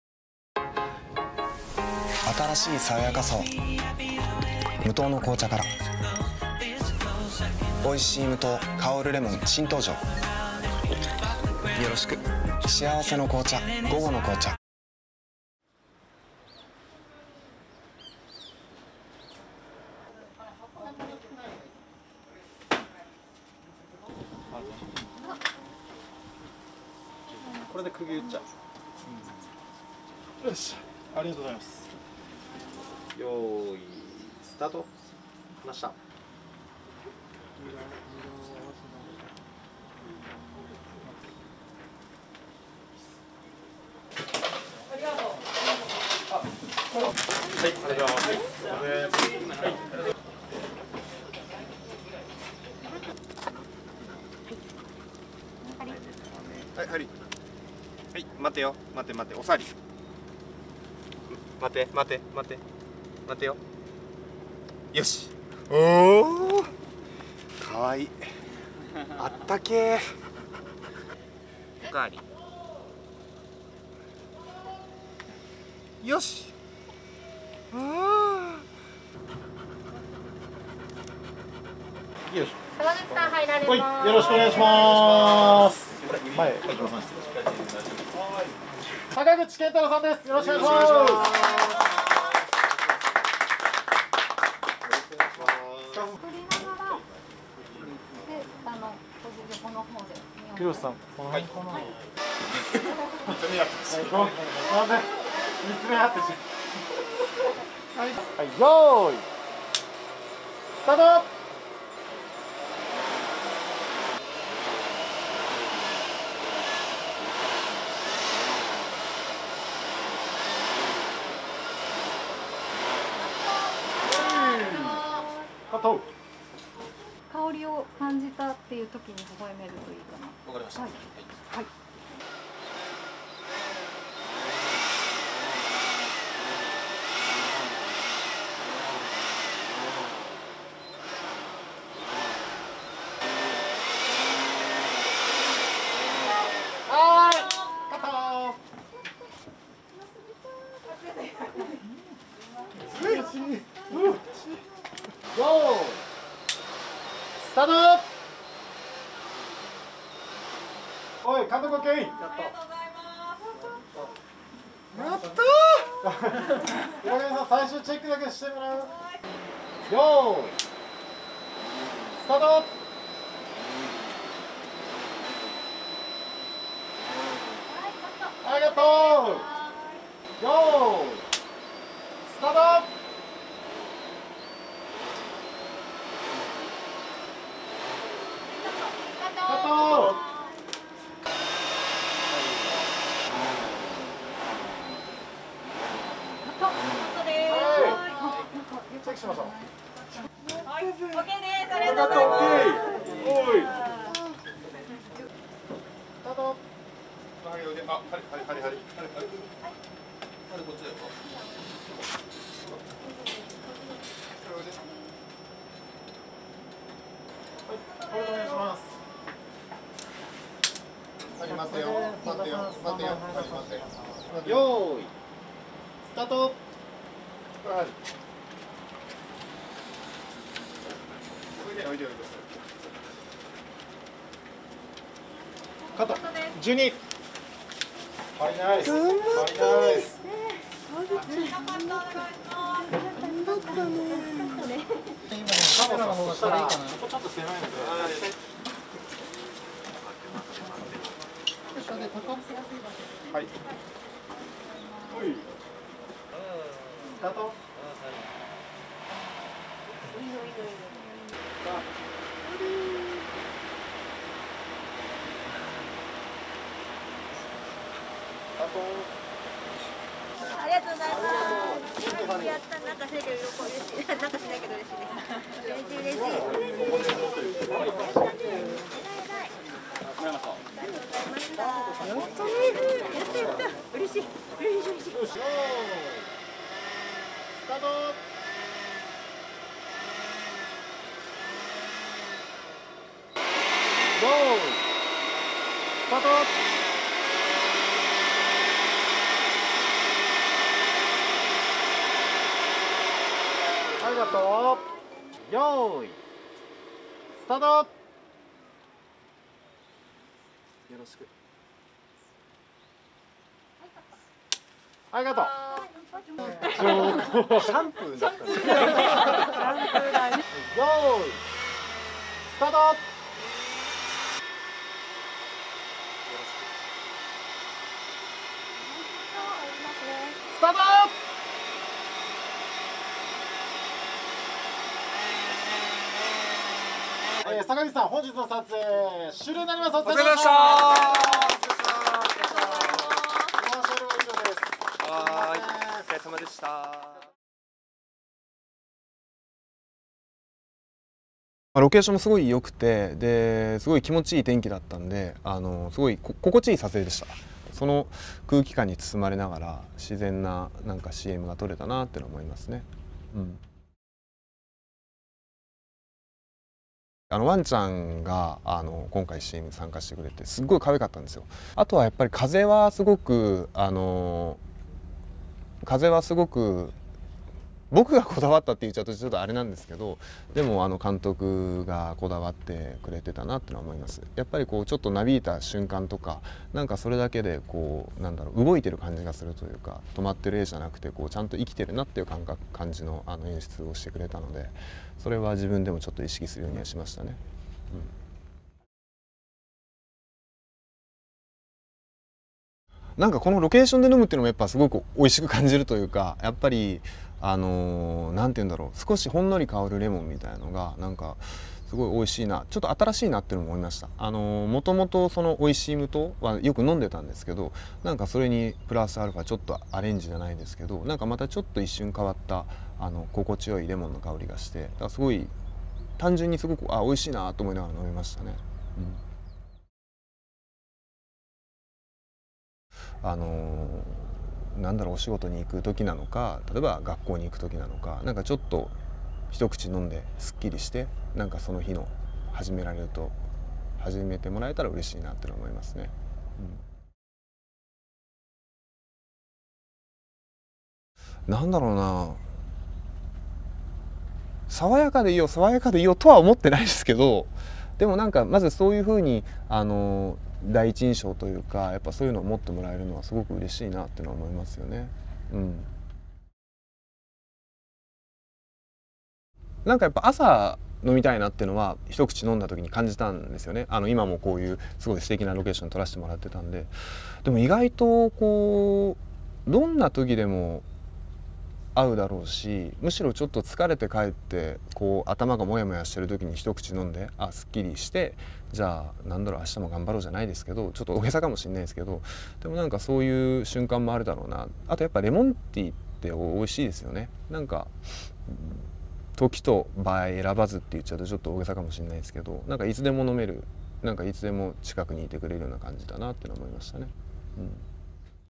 坂口健太郎、レモン片手に爽やか笑顔 愛犬とともに心地よい時間 「キリン 午後の紅茶 おいしい無糖 香るレモン」新CM＆メーキング
俳優の坂口健太郎さんが出演する「キリン 午後の紅茶 おいしい無糖 香るレモン」の新CMが9月28日、公開された。同日から放送。

　CMは「新しい爽やかさを、無糖の紅茶から。」編。目の前に海を一望できる窓辺で、「無糖紅茶×レモン」が織りなす“新しい爽やかさ”を、坂口さんが“爽やか”に実感しながら愛犬とともに心地よい時間を楽しむ姿を描いている。メーキング、インタビューも公開された。